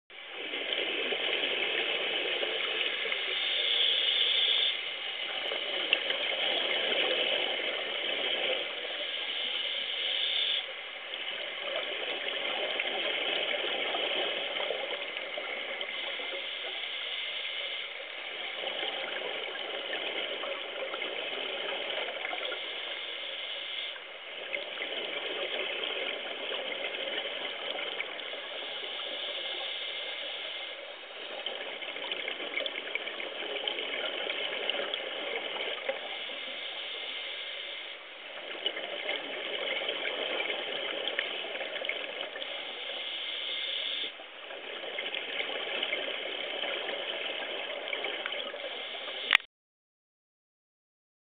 Turtle while Scuba diving Puerto Aventuras
Turtle while Scuba diving Puerto Aventuras on 1/28/2010 shot with Sea & Sea DX-1200HD